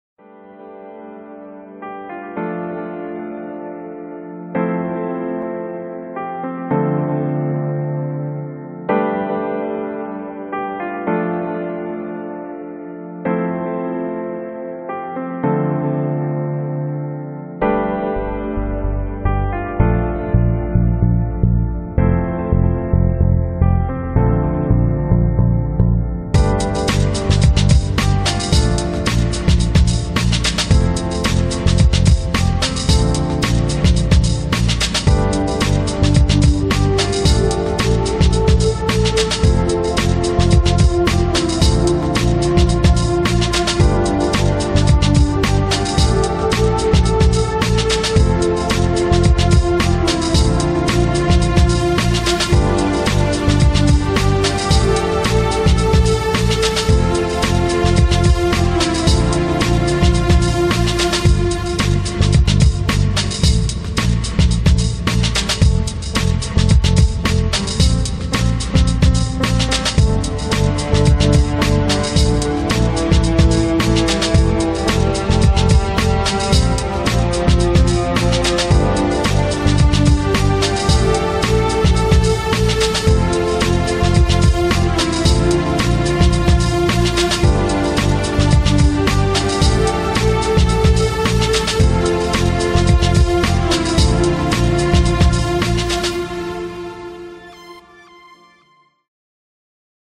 Timelapse Cam 1 - Natt til 4. april 2018
Været i Moss natt til onsdag 4. april 2018.